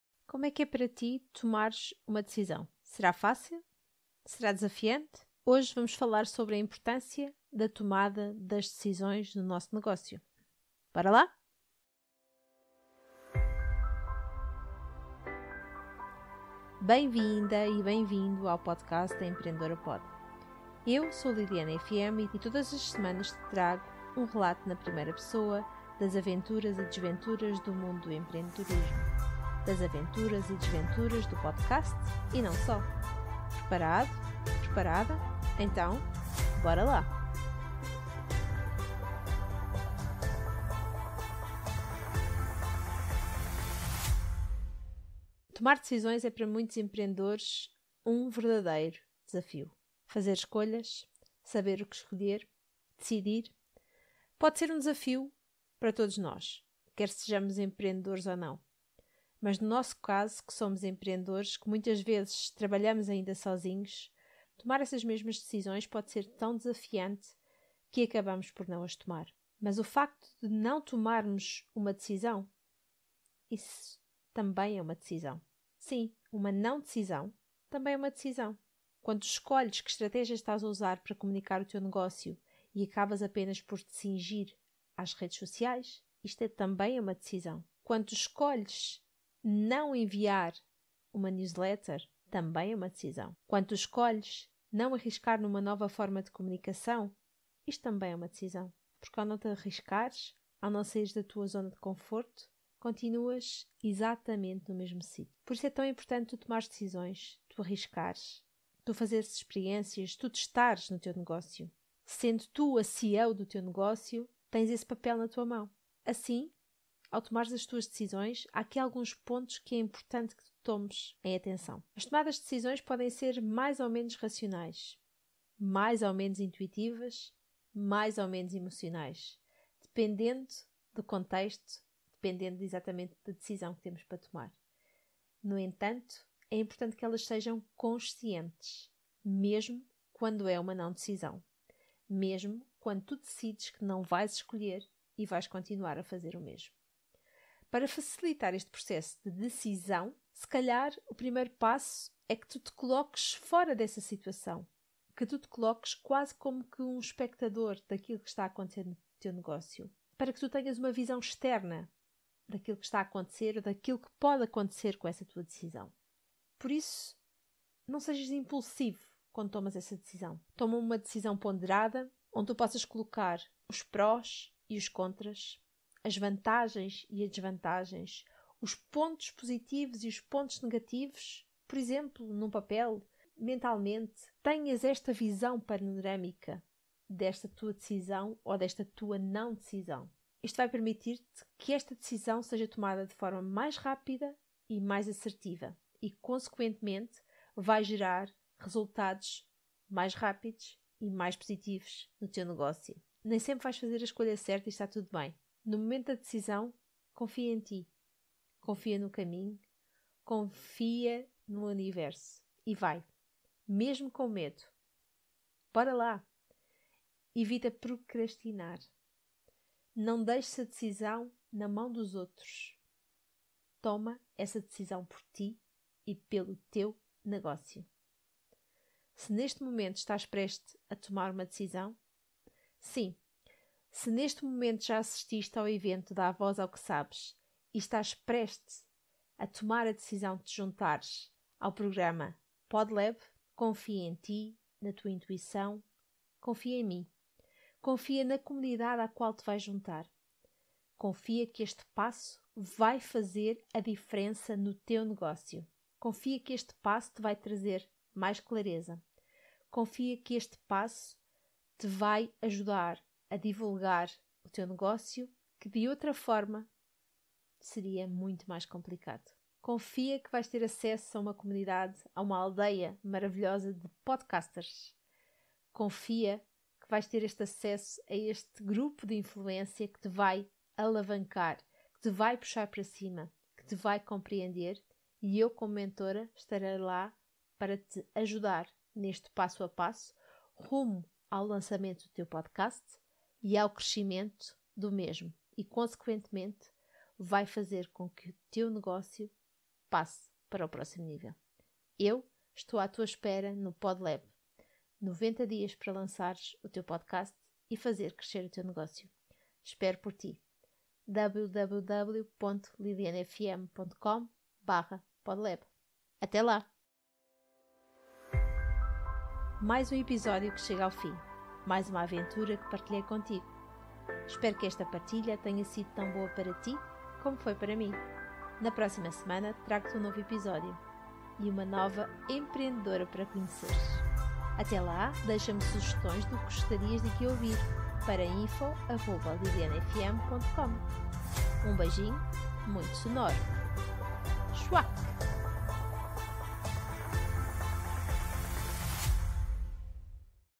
A Empreendedor@ PODe - A importãncia da tomada de decisões no nosso negócio
Uma das  tarefas mais desafiantes num negócio é tomar decisões.

Tomar decisões é uma tarefa de grande responsabilidade e pode trazer consequências que podem ser até o teu pior pesadelo.

Por outro lado, quando feitas de forma bem pensada e com um planejamento sólido, as tomadas de decisão podem levar-te a alcançar os melhores resultados.

Tomar decisões no nosso negócio pode ser desafiante mas é super importante e é disto que te falo neste episódio.